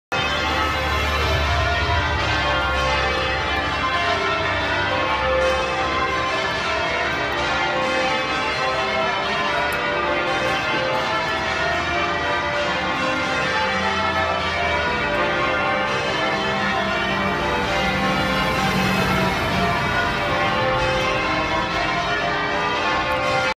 Bow Bells
The bells of St Mary-le-Bow on London's Cheapside. They are loud!